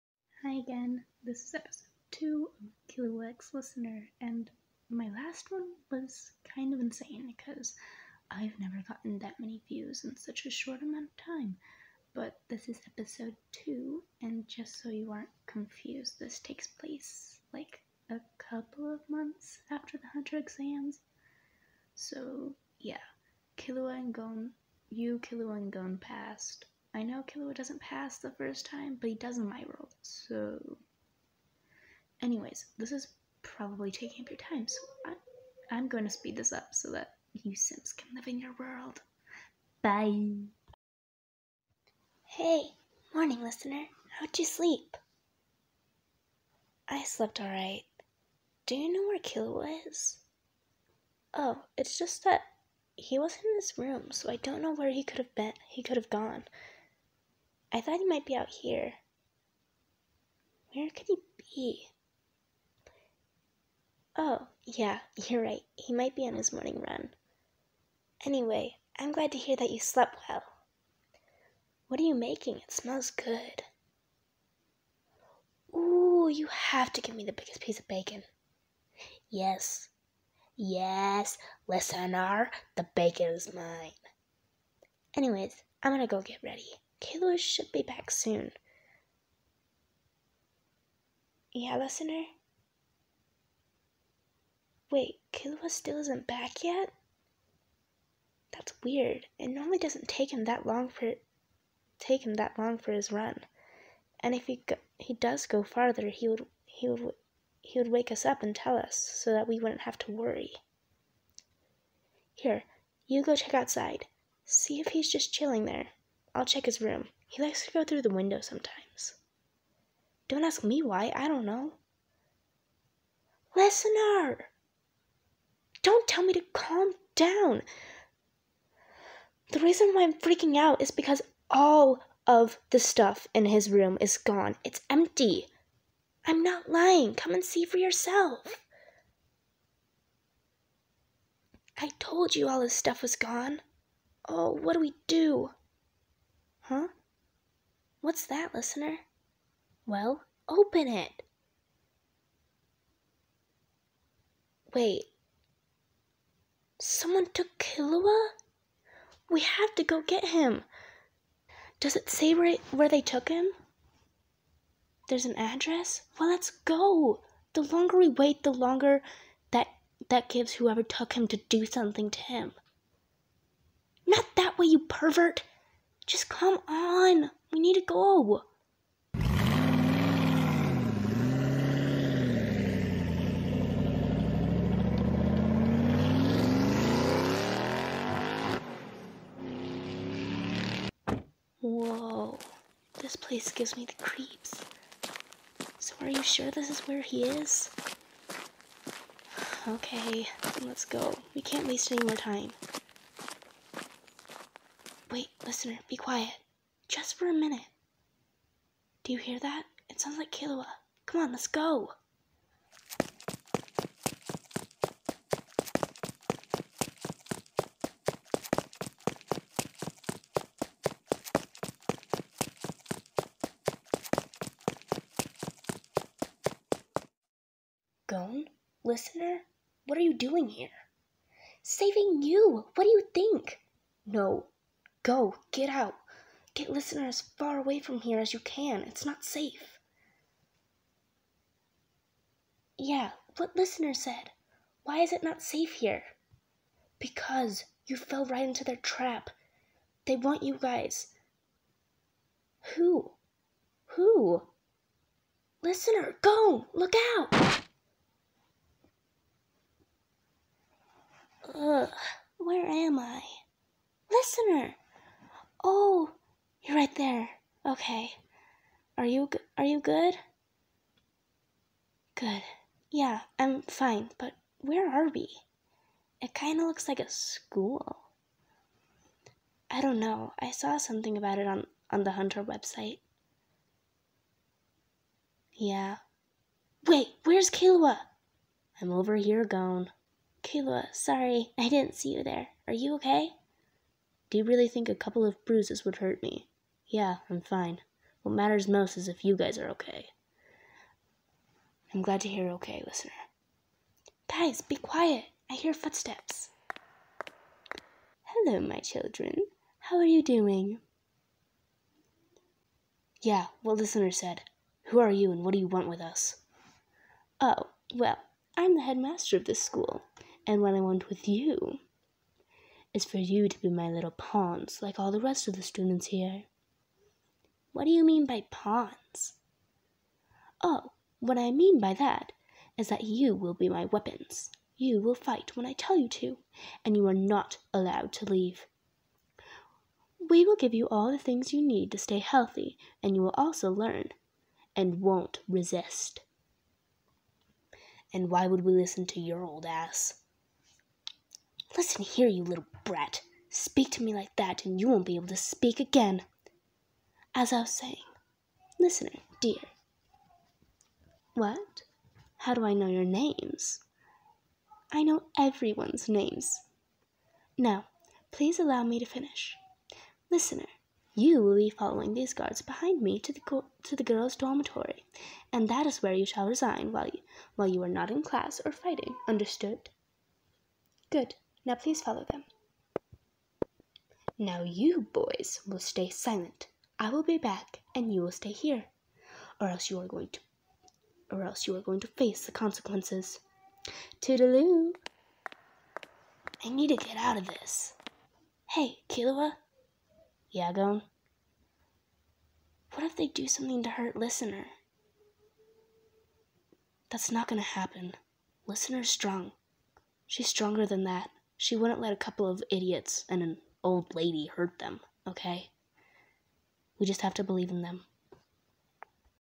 Killua x Listener EP.2
I COULDN'T HELP MYSELF FROM MAKING A PART TWO I AM HOME ALONE SO WHAT BETTER THING TO DO THAN MAKE ANOTHER ONE I THINK I AM STARTING TO BECOME OBSESSED ALSO I AM GOING TO DO A CIEL PHANTOMHIVE SERIES BECAUSE I AM MUCH BETTER AT IMPERSONATING HIM SOOOOO YEAH LOOK FORWARD TO THAT!!!😍